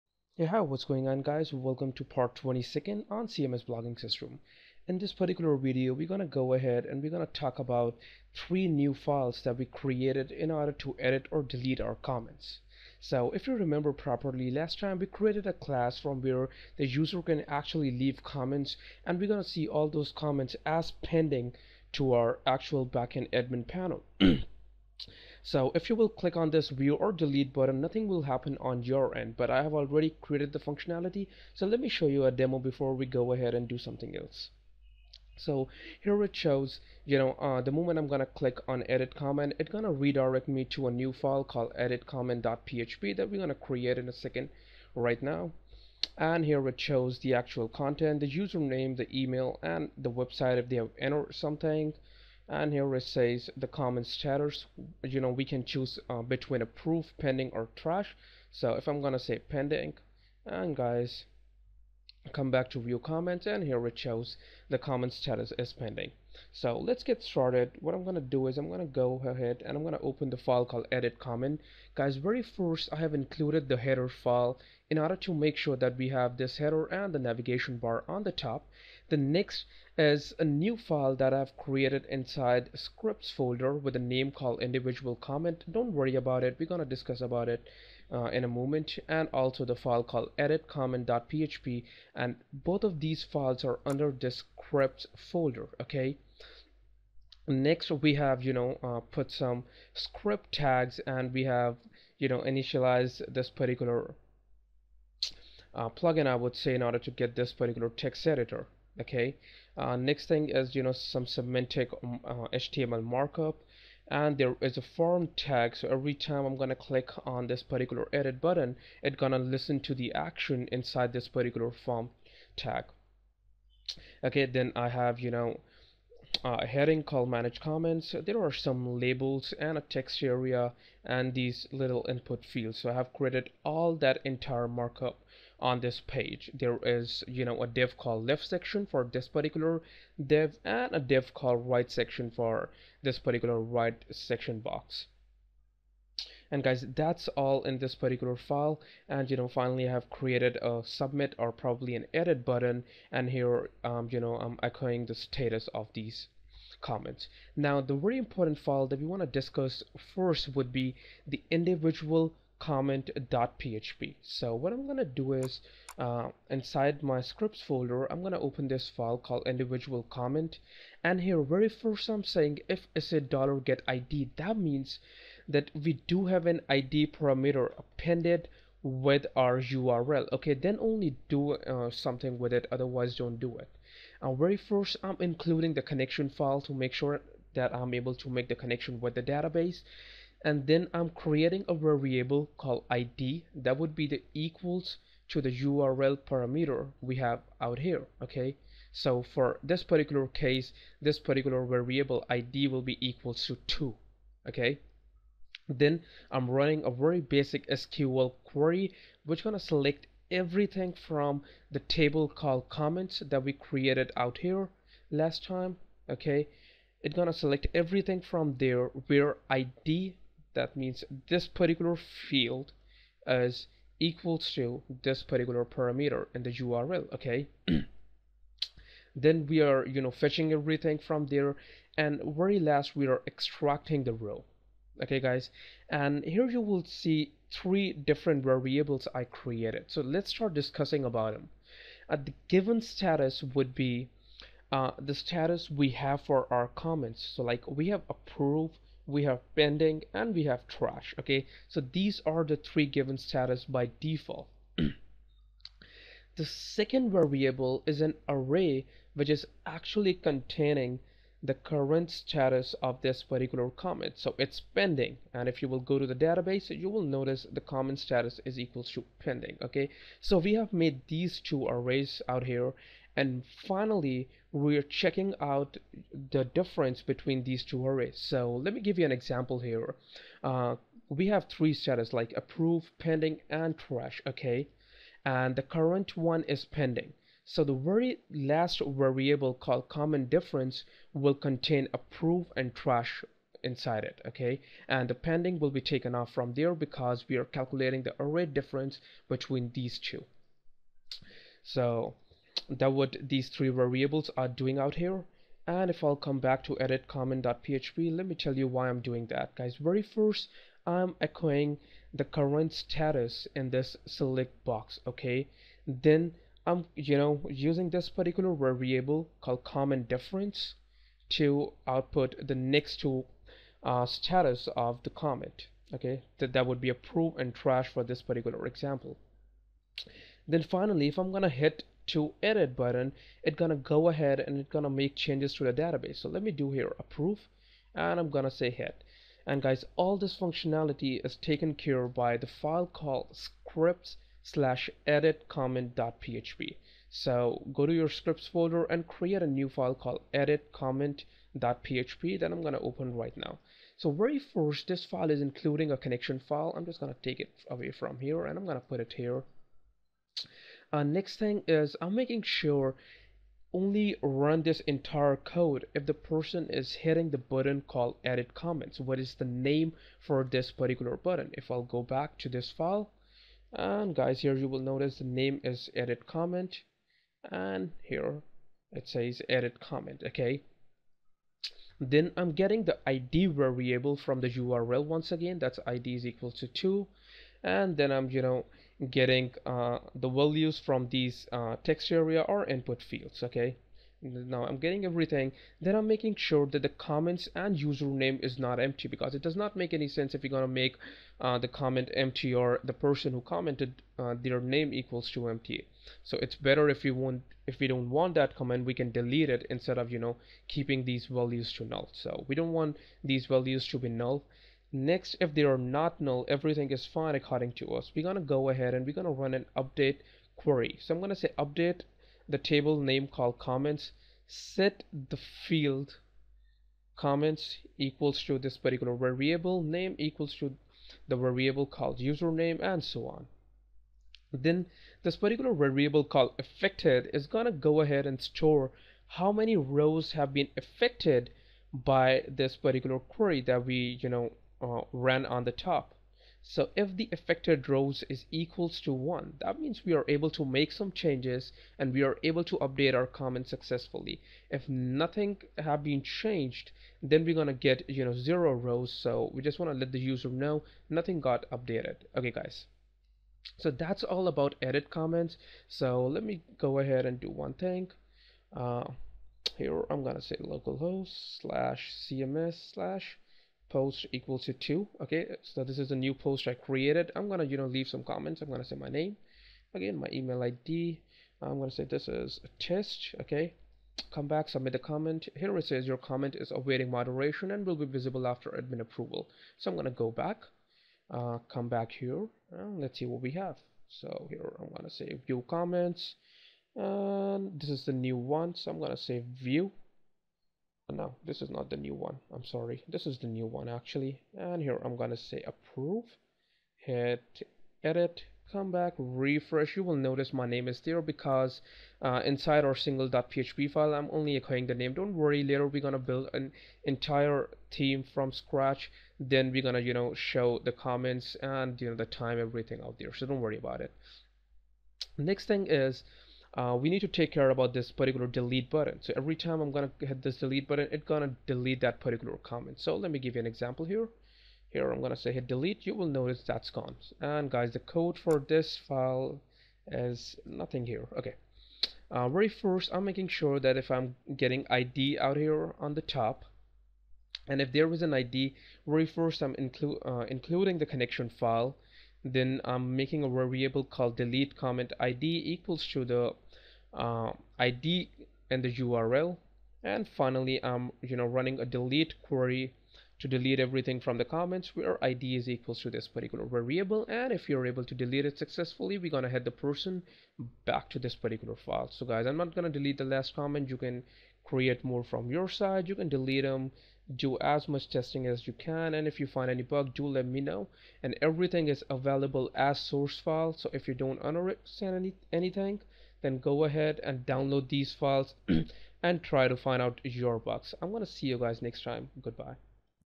CMS Blogging System Part 22
This video will take you through to add edit and delete functionality to your commenting system